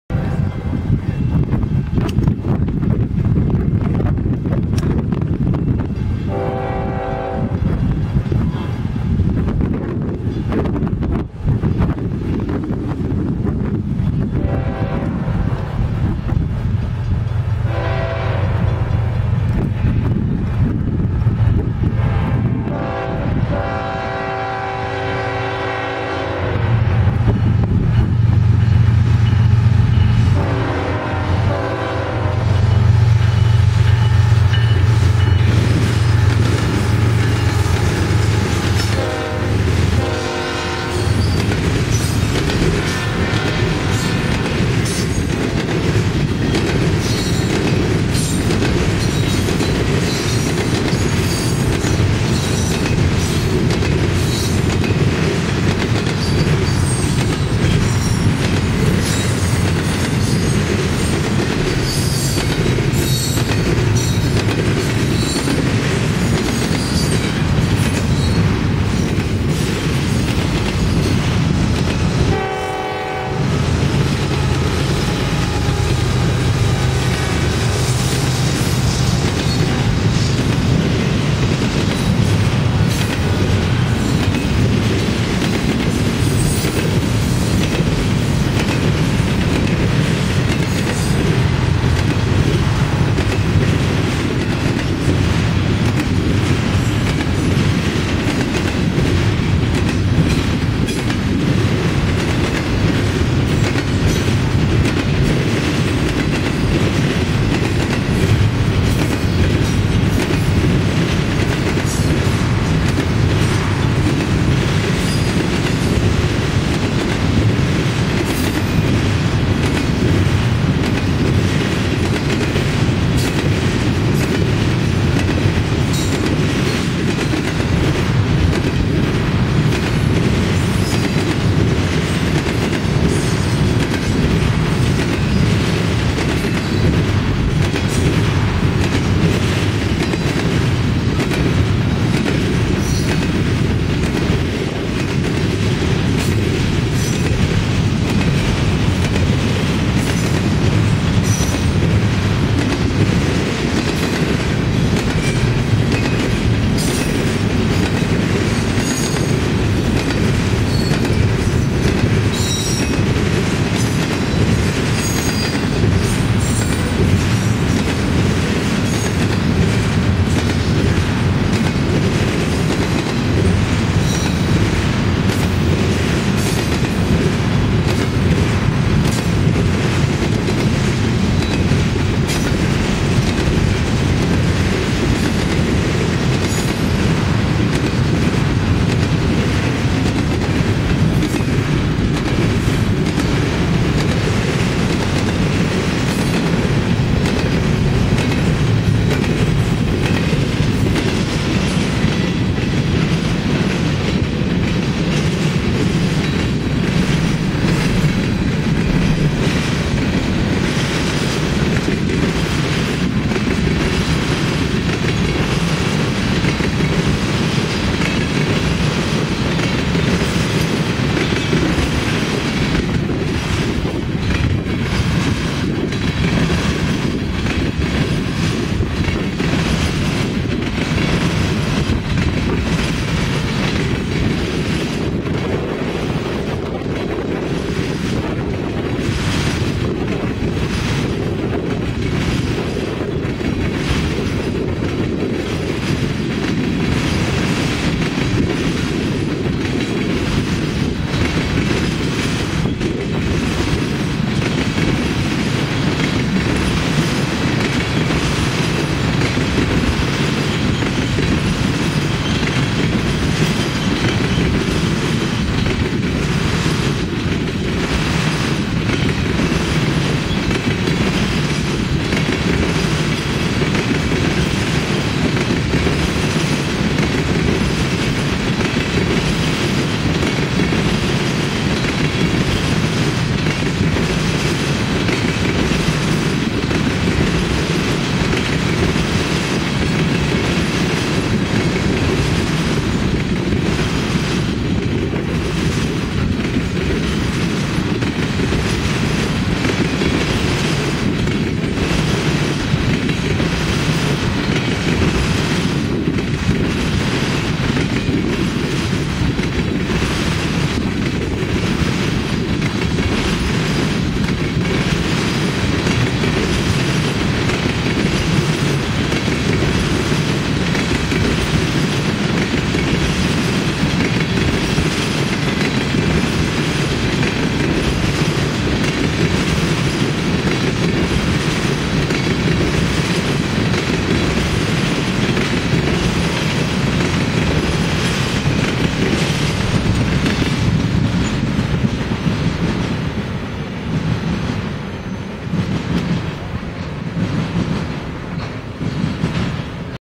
Deshler, Ohio railfanning Pt.5 (Train Race)
2 trains. . . determination, THE MUTHA TRUCKIN BEANZ, and a Diamond, Who. . Will. . . WIN!! lol